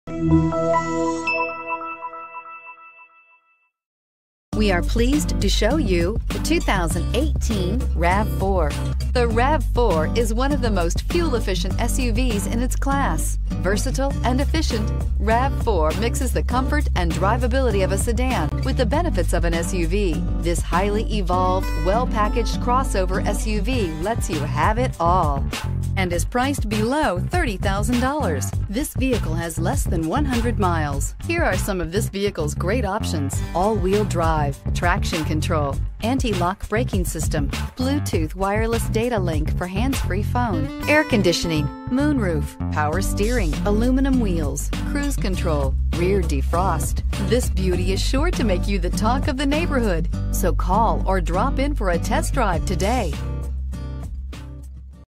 2018 Toyota RAV4 XLE Maplewood, St Paul, Minneapolis, Brooklyn Park, MN J13543
Maplewood Toyota
2873 Maplewood Dr

Maplewood Toyota
2873 Maplewood Drive

Pick up this Silver Sky Metallic 2018 Toyota RAV4, available today at Maplewood Toyota. This could be the one you've been searching for.